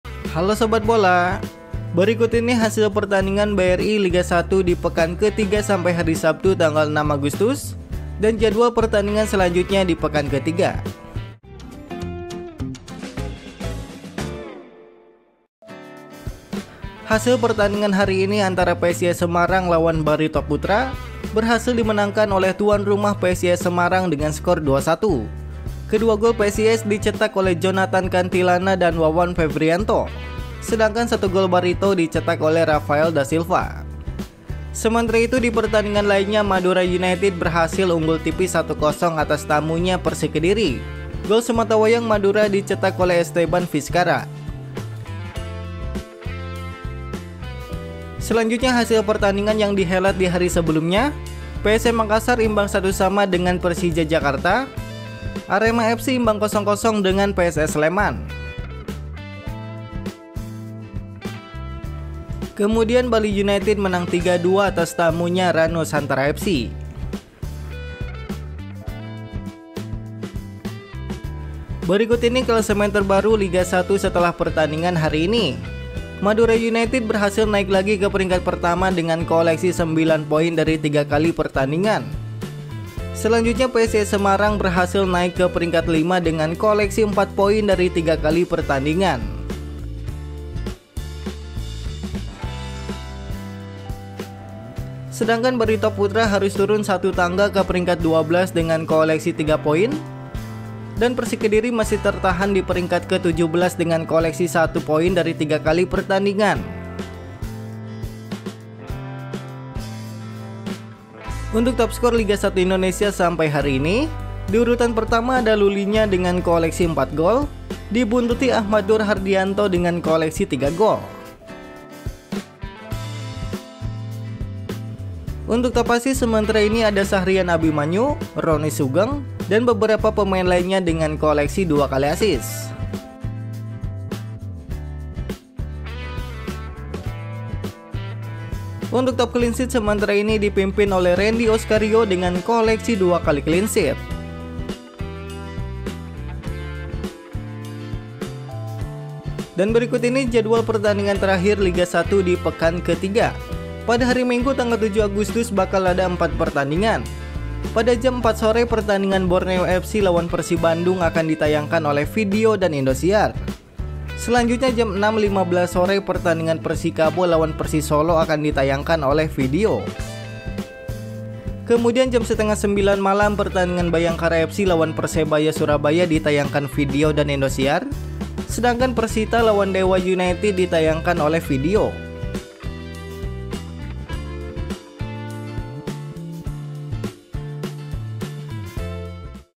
Hasil Liga 1 Hari Ini - Madura United vs Persik, PSIS vs Barito | Klasemen BRI Liga 1 2022-2023
Hasil Liga 1 full time Madura United vs Persik Kediri 1-0, PSIS Semarang vs Barito Putera 2-1 BRI Liga 1 2022/2023 pekan 3 Sabtu 6 Agustus 2022, Update Klasemen Liga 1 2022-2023 Terbaru, Topskor dan Jadwal Liga 1 hari ini Live Indosiar Borneo fc vs Persib Bandung

Hasil Liga 1 hari ini:
ull time PSIS Semarang vs Barito Putera 2-1
full time Madura United vs Persik Kediri 1-0
full time Arema FC vs PSS Sleman 0-0
full time PSM Makassar vs Persija Jakarta 1-1
full time Bali United vs Rans Nusantara FC 3-2

Jadwal Liga 1 selanjutnya:
Borneo FC vs Persib Bandung Live Indosiar, Vidio
Persikabo 1973 vs Persis Solo Live Vidio
Bhayangkara vs Persebaya Surabaya Live Indosiar, Vidio
Persita vs Dewa United Live Vidio